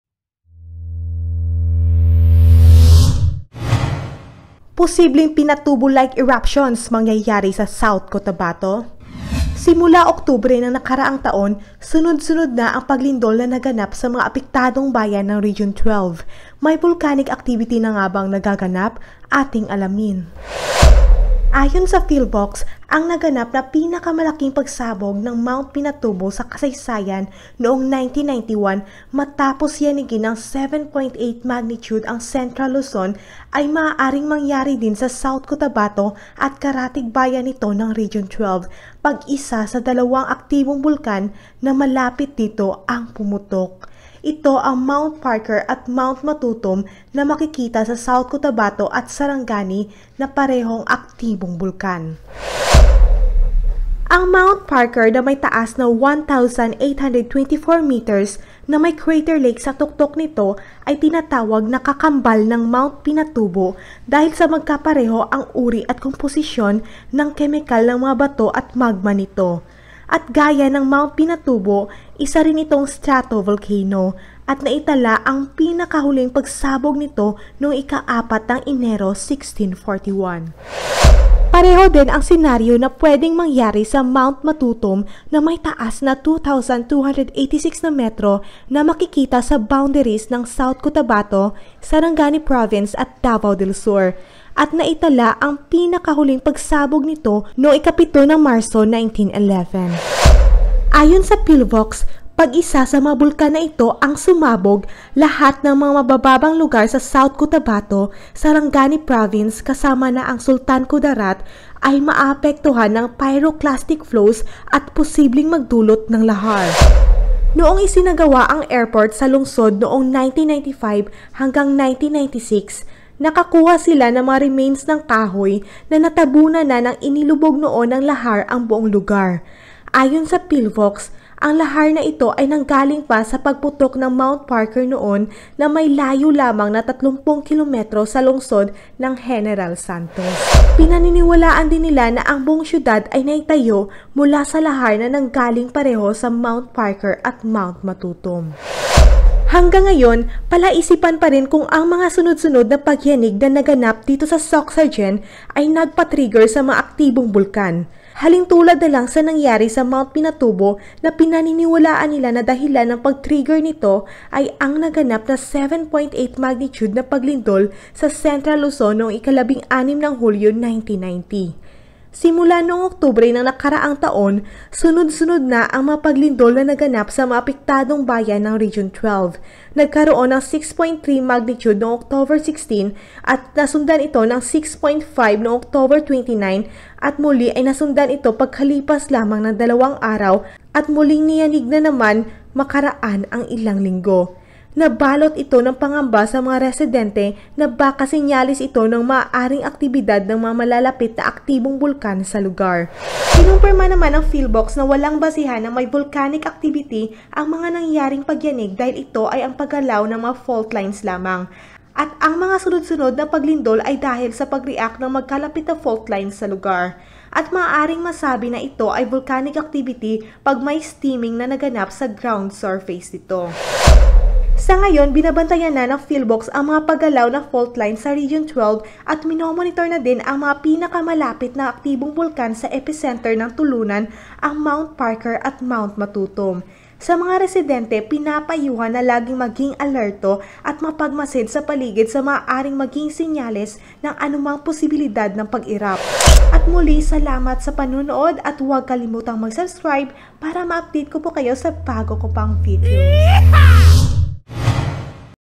Matutum & Mt Parker Delikado pag ma Trigger ng mga PagLINDOL!
Simula octobre ng ankaraang taon , sunod sunod na paglindol ang naganap sa mga apektadong bayan ng region 12 . , may volcanic actiitivty na nga bang nagaganap?
Ayon sa philvocs ang naganap na pinakamalaking pagsabog ng mt pinatubo sa kasaysayn  nuong 1991   matapos yanigin ng 7.8 magnitude ang central luzon ay maaring mangyari din sa south cotabtao  at karatig bayan  nito ng region 12 pag isa sa dalawang aktibong bulkan na malapit dito ang pumutok.
Ito ang Mt. Parker at Mt. Matutumm na makikita sa south cotabto at sarangani na parehong aktibong bulkan.

Ang Mt parker , na may taas ng 1, 824 meters na may crater lake sa tuktok nito   ay tinatawag na kakambal  ng Mt pinatubo dahil sa magkapareho ang uri at komposisyon ng kemikal ng mga bato at magma nito.
at gaya ng mt. pinatubo, isa rin itong stratovolcano at Naitala ang pinakahuling pagsabog  nuong ikaapat ng enero, 1641.

Ang Mt. Matutum na may taas ng 2,286 metro na makikita sa boundaries ng south cotabato, sarangani province at davao del sur. At naitala ang huling pagsabog nito nuongh ika pito ng marso , 1911.

Ayon sa philvocs, poag isa sa mga bulkan na ito ang sumabog, lahat ng mga mababang lugar sa south cot.,  ,  sarangani kasama na ang sultan kudarat  ay maapektuhan ng pyroclastic flows., at posibleng magdulot ng lahar .